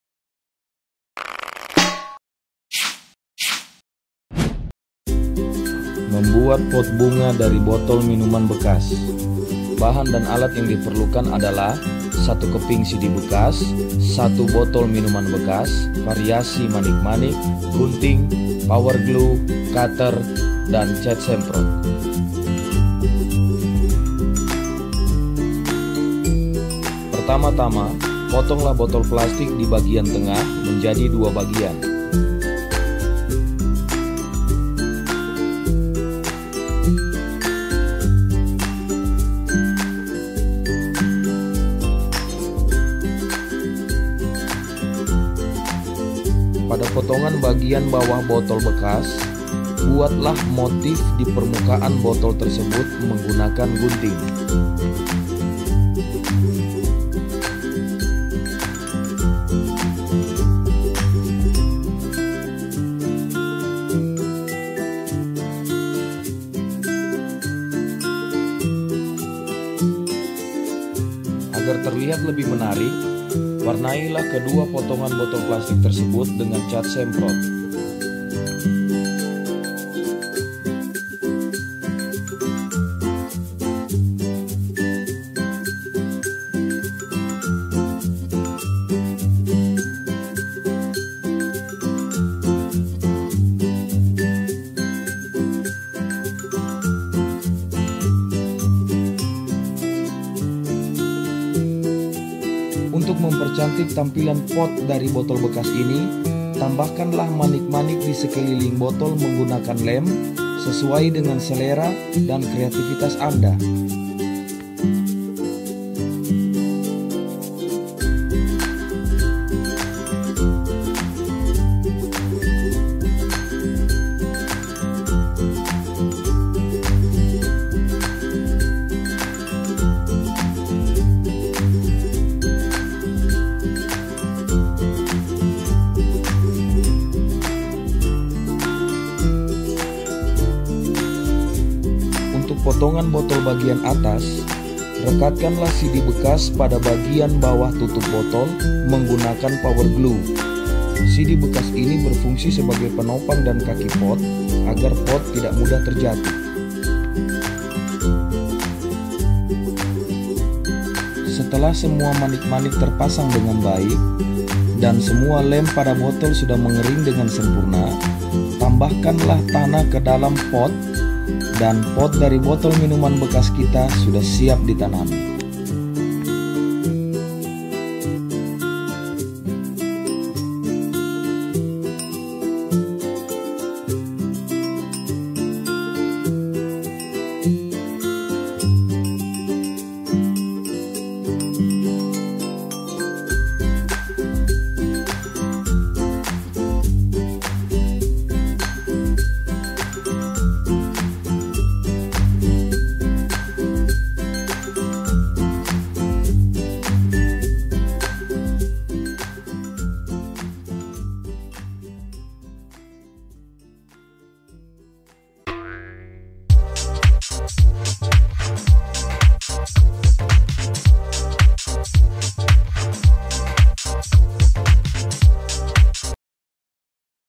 Membuat Pot Bunga dari Botol Bekas (pot bunga kreatif)
Kali ini Tobikin.com akan memberi contoh cara membuat vas bunga atau pot bunga.

Bahan-bahannya sederhana. Dapat dengan mudah ditemui di rumah.

Pot bunga kreatif kerajinan tangan ini bisa kamu buat dengan barang-barang bekas di rumah, seperti botol bekas, dll.

Nah untuk jelasnya silakan lihat dalam video ya..